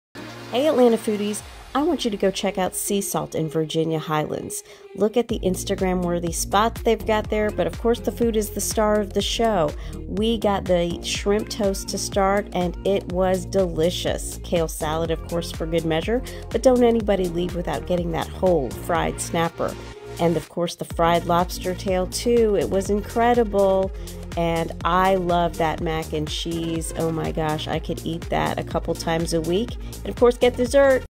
Fryday Feast: Lobster Tails, Fried Snapper, and Shrimp Toast with Jerk Sauce
Join us for a Fryday feast and indulge in delicious seafood delights! Choose between succulent lobster tails or crispy fried snapper. Start off with a mouth-watering shrimp toast appetizer topped with spicy jerk sauce. Don't forget to snap a pic by the Instagram wall and share your Fryday experience with us.